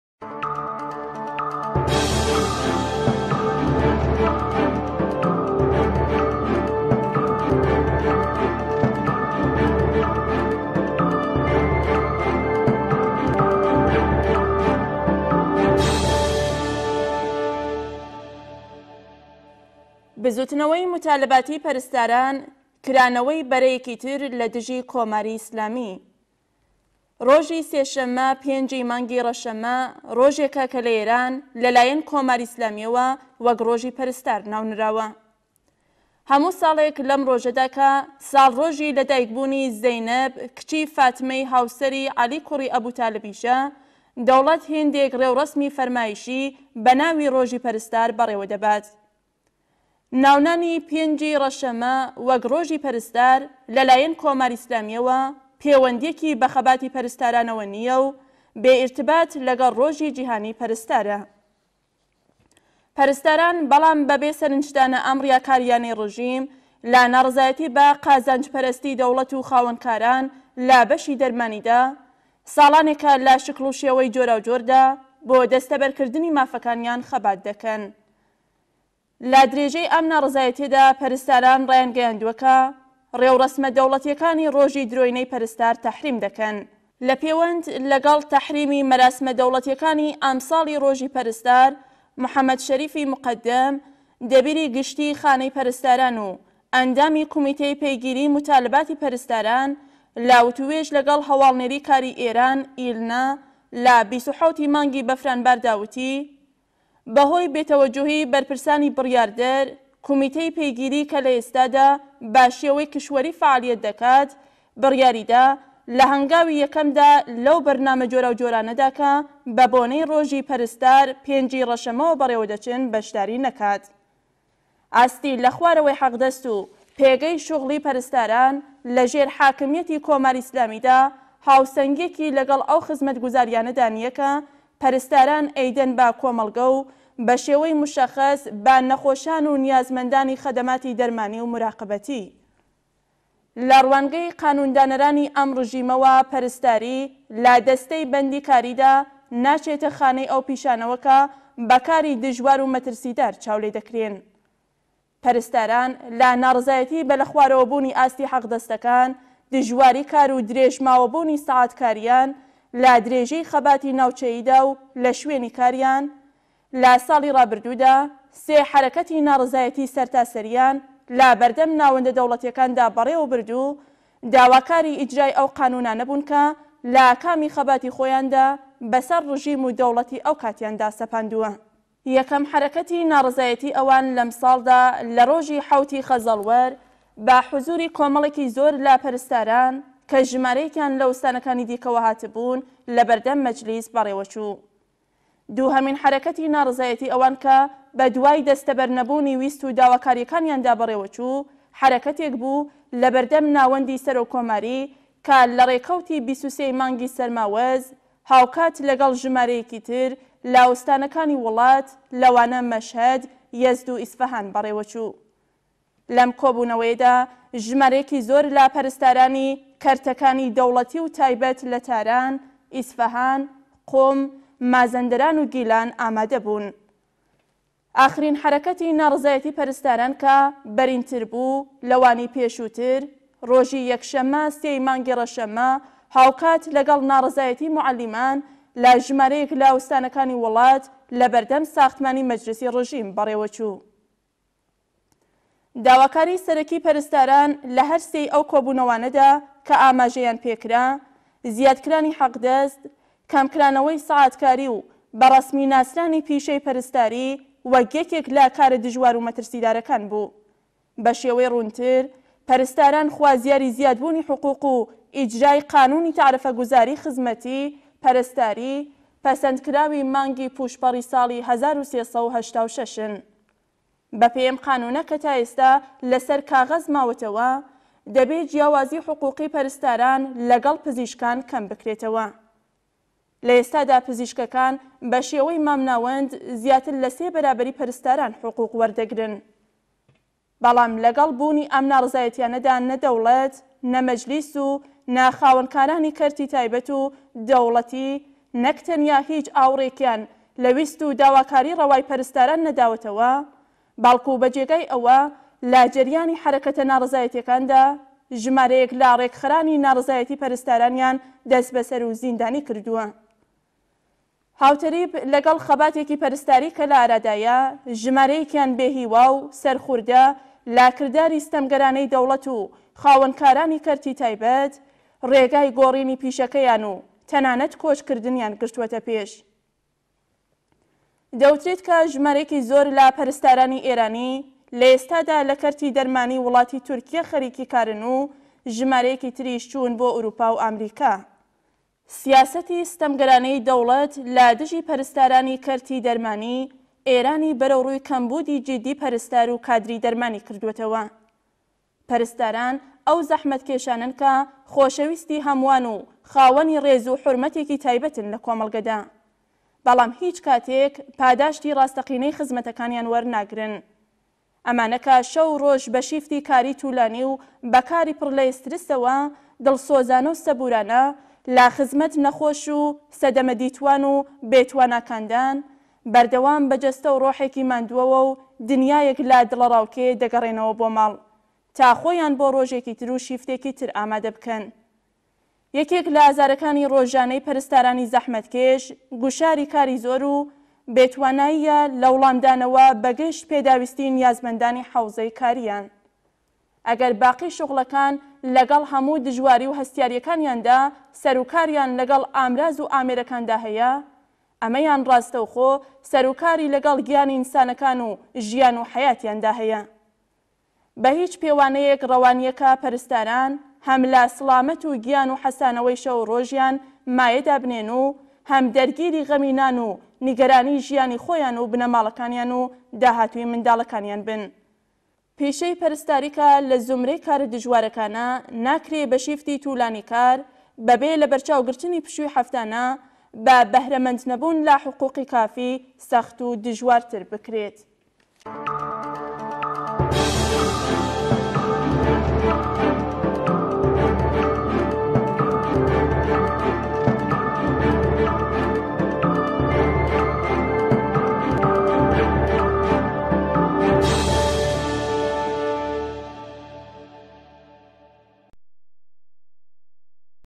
سخن روز
جنبش مطالباتی پرستاران، گشودن جبهه دیگری علیه جمهوری اسلامی